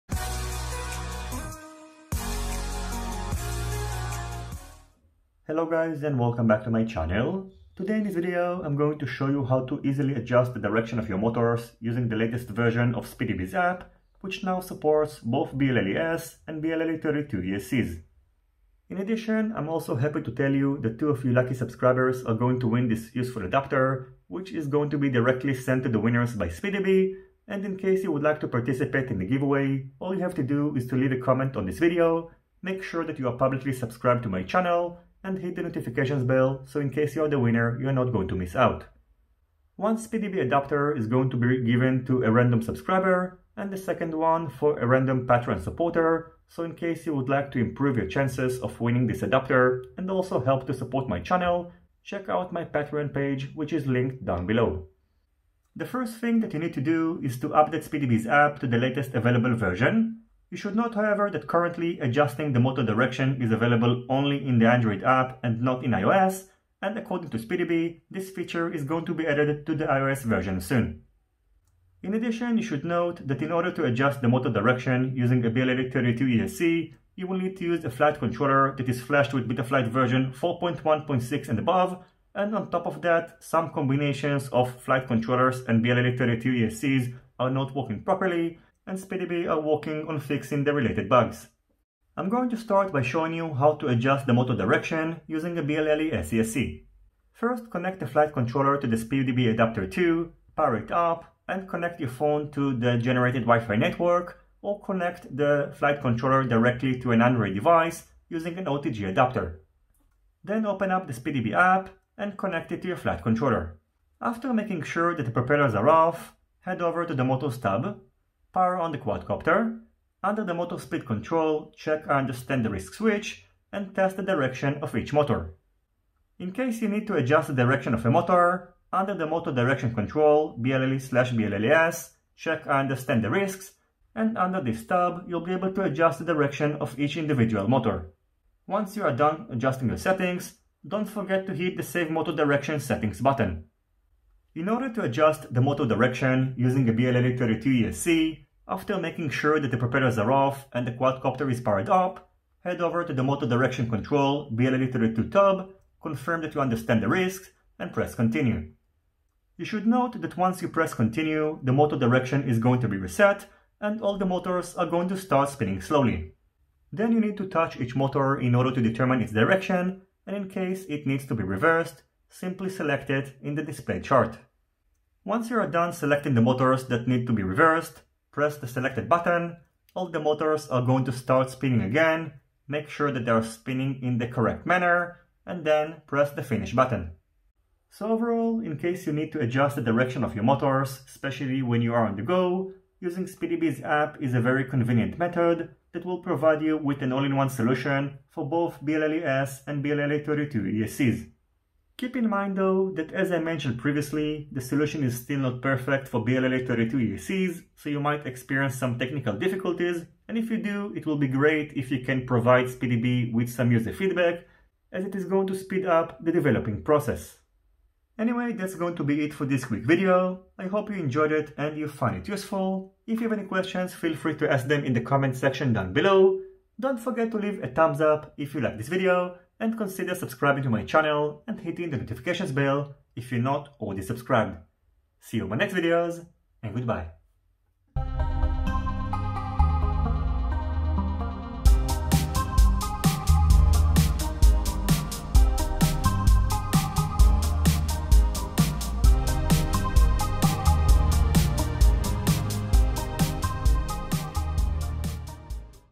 Easily Adjust Motors Direction Using SpeedyBee App (BLHELI_S & BLHELI_32)
╠╗║╚╝║║╠╗║╚╣║║║║║═╣ I Really Appreciate Your Support!